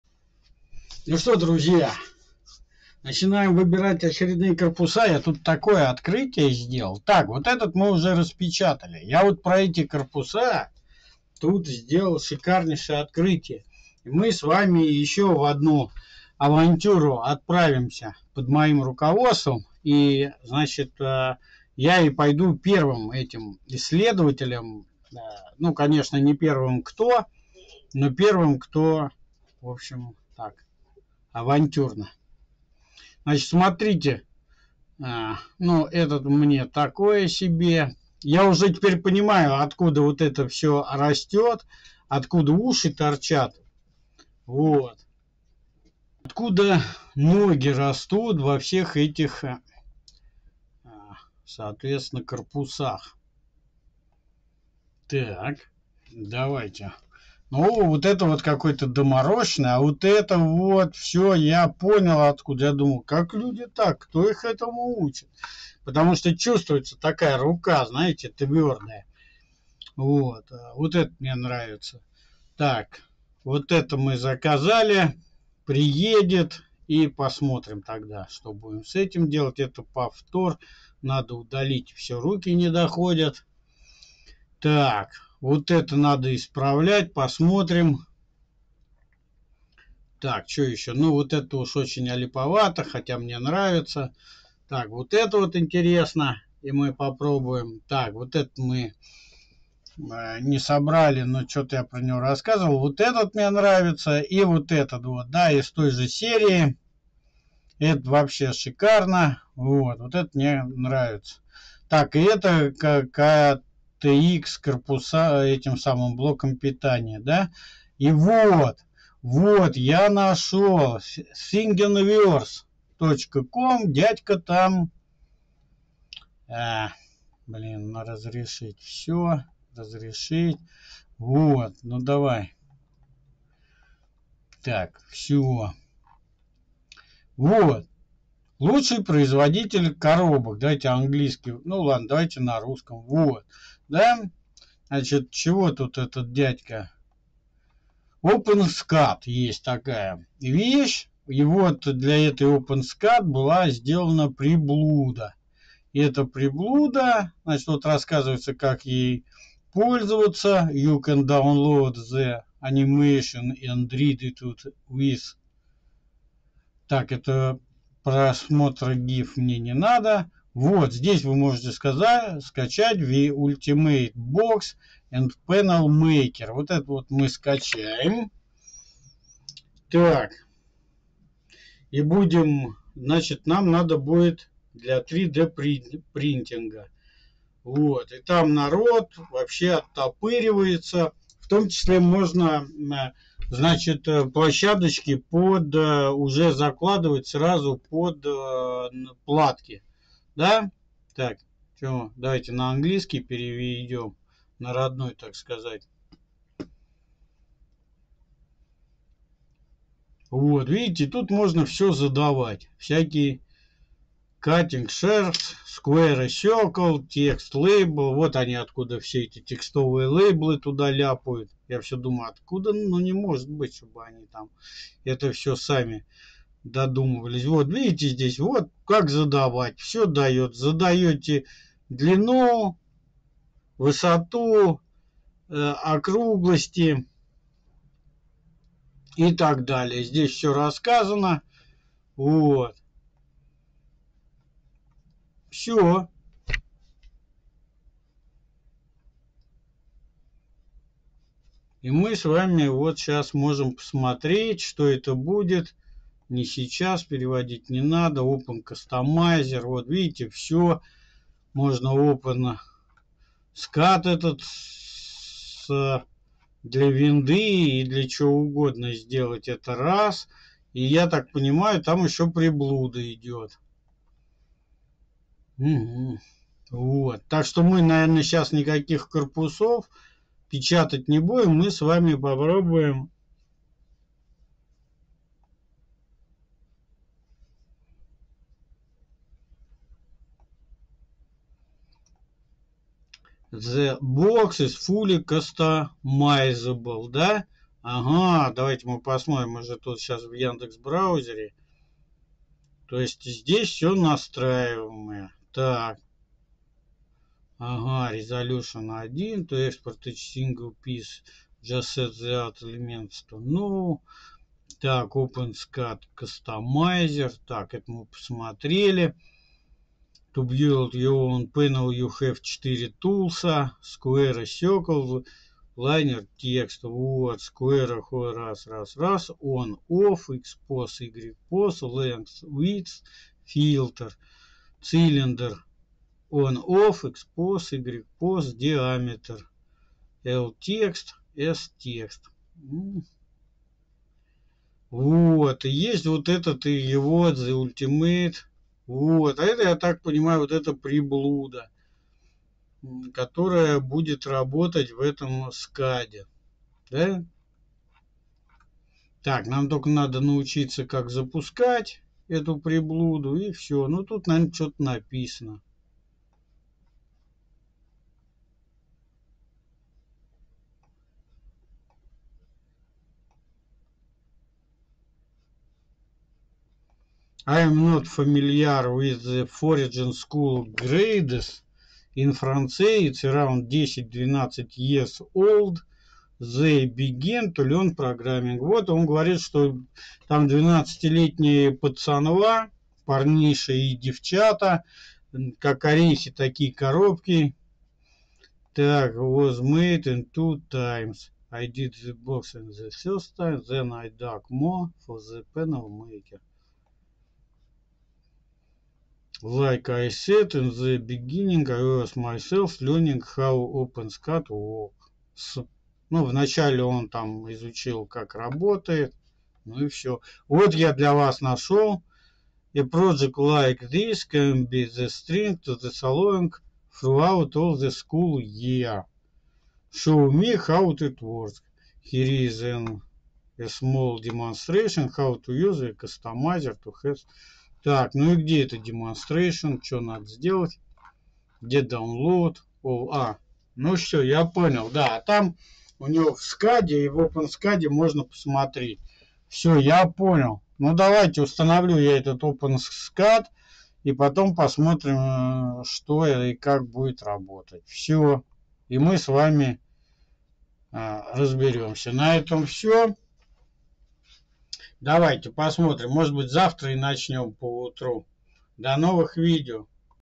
Выбирая корпус нашел программу как делать корпуса и программу OpenScad! 2024 01 14 10 42 48
Продолжение следует...